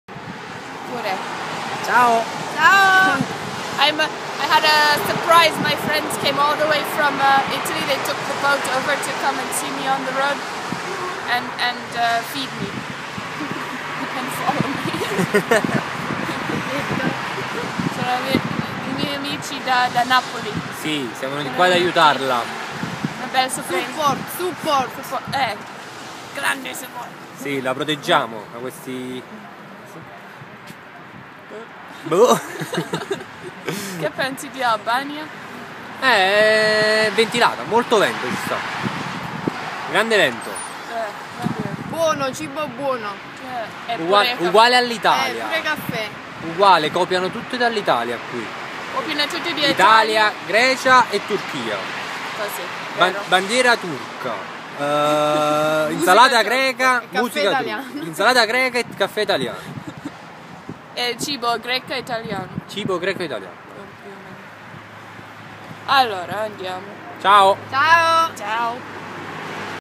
A friendly surprise
When my friends from Italy take the boat over to Albania to join me on the road a couple days.